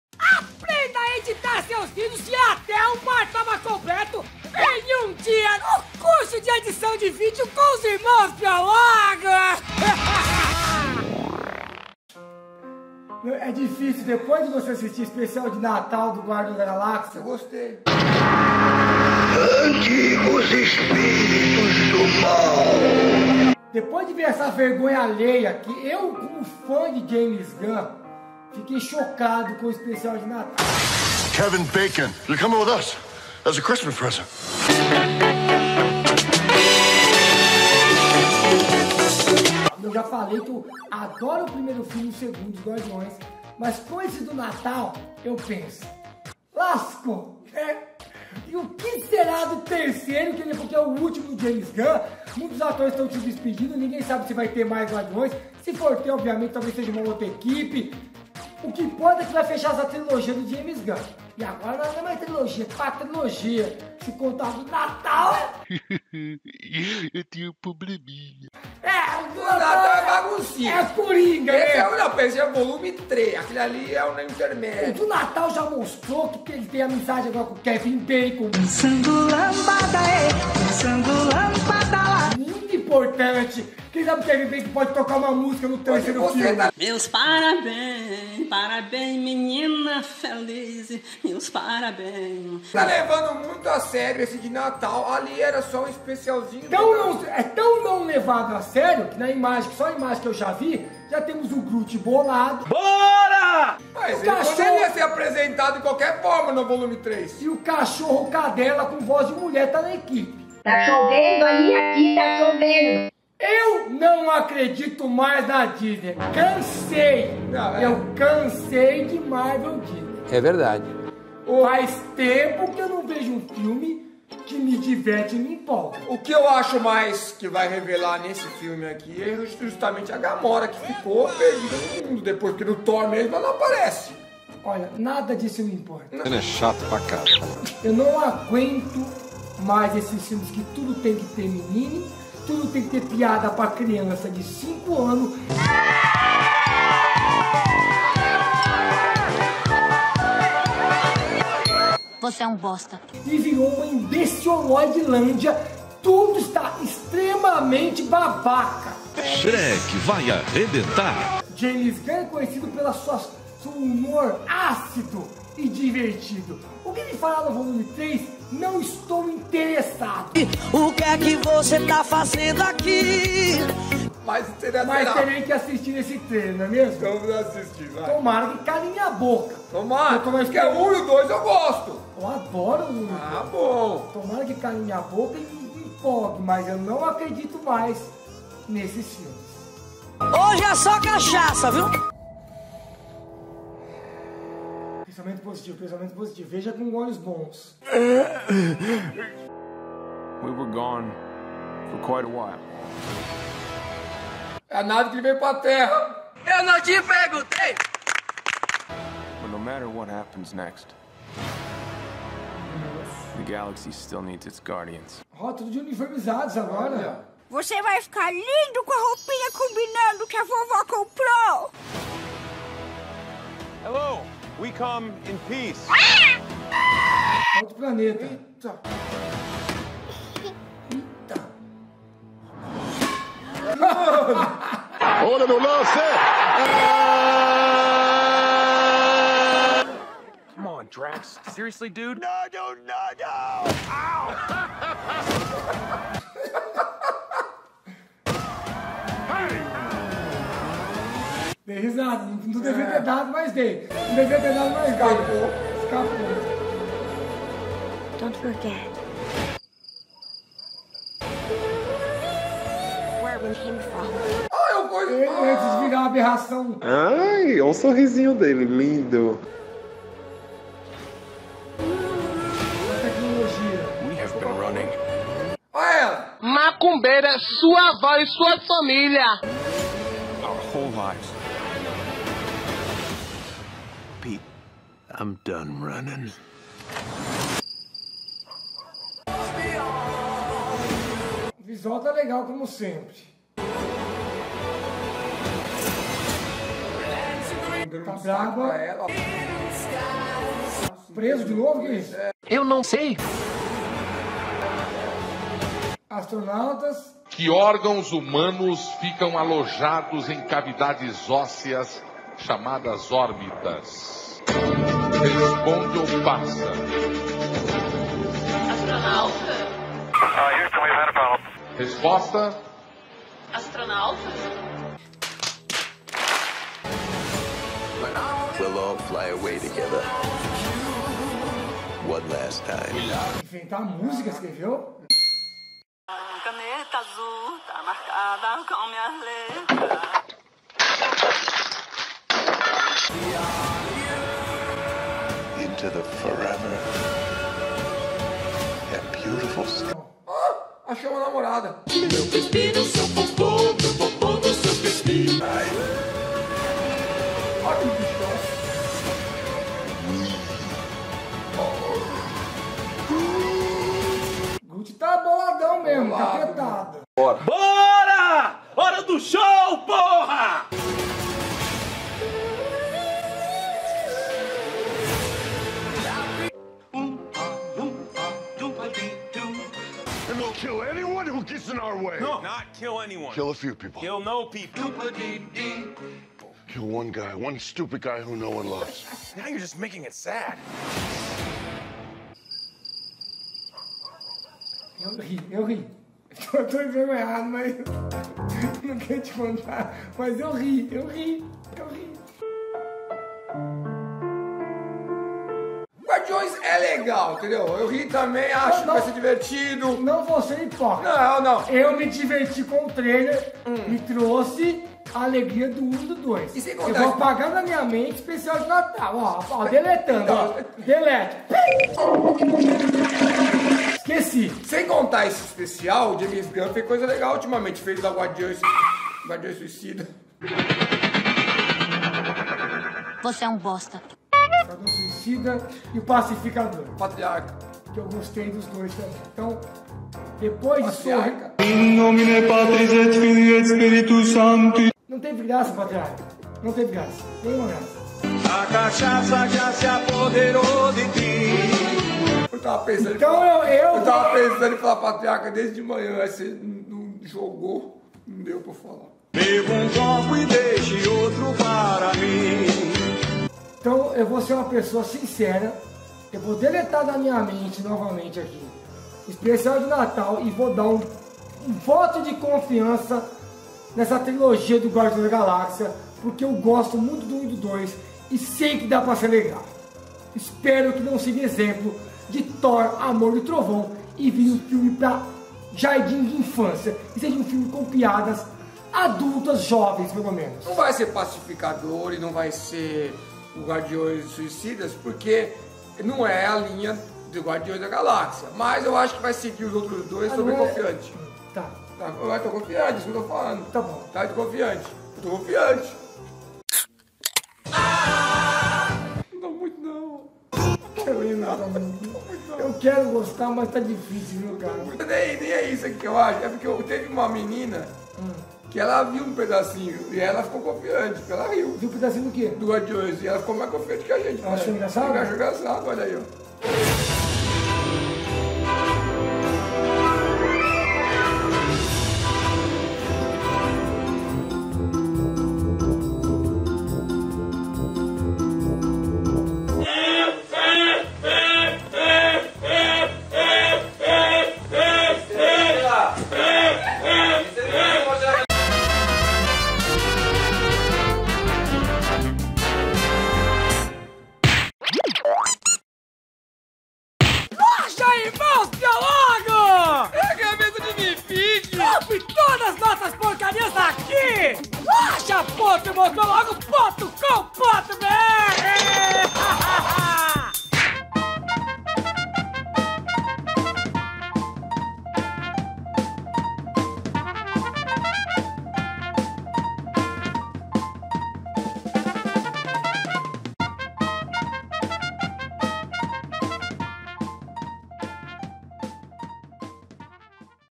GUARDIÕES DA GALÁXIA VOL. 3 Há uma Esperança de um FILME BOM?
Depois do especial de Natal INFANTIL dos Guardiões, será que da pra acreditar que esse filme se manterá tão legal quanto aos anteriores?
GUARDIÕES DA GALAXIA VOL. 3
Peter Quill, ainda se recuperando da perda de Gamora, deve reunir os Guardiões da Galáxia em uma missão para defender o universo, além de proteger um dos integrantes do grupo.

☻►Quer Mandar algo? Mande aqui:
Irmãos Piologo
Caixa Postal 622
Indaiatuba-SP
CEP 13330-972